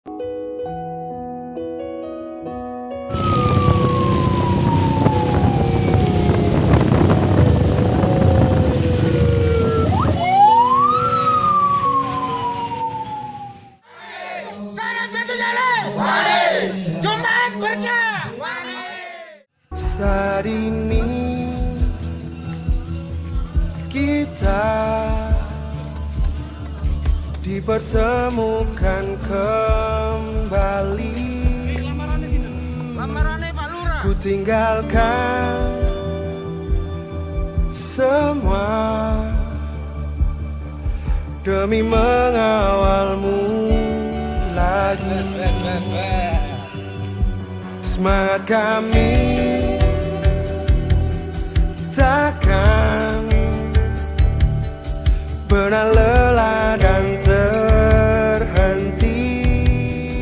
Konvoi Rombongan Bonek Bonita ke Panti Asuhan Al Hidayah Krian Junat Berkah 30 Oktober 2020
Video dari Siti Rafika Hardhiansari💚